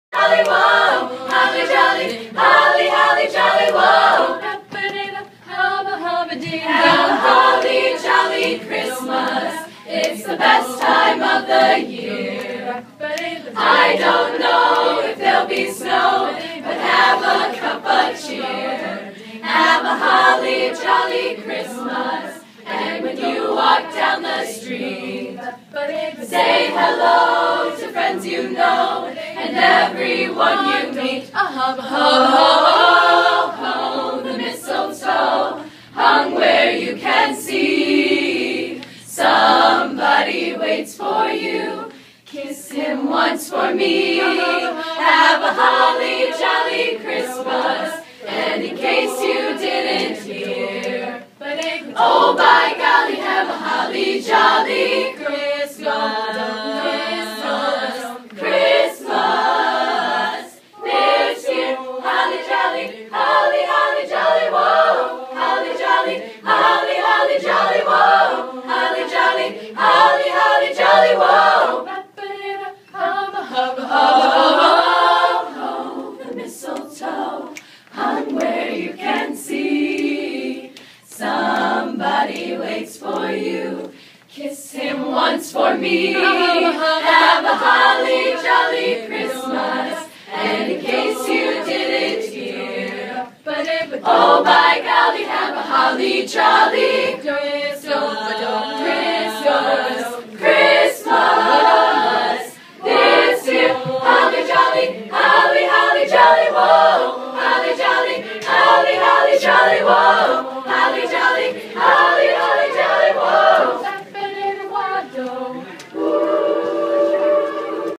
DGN Treble Choir Office Drive-By Caroling
Members of the DGN Treble Choir surprised Mr. Teague this morning with a wonderful holiday caroling drive-by.